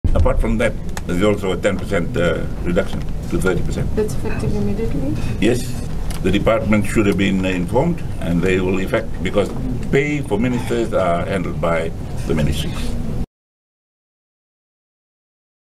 THE FIJI TIMES | 30% pay cut for all ministers
All ministers in The People’s Alliance-National Federation Party-Social Democratic Liberal Party coalition Government have taken a 30 per cent pay cut, says Prime Minister Sitiveni Rabuka.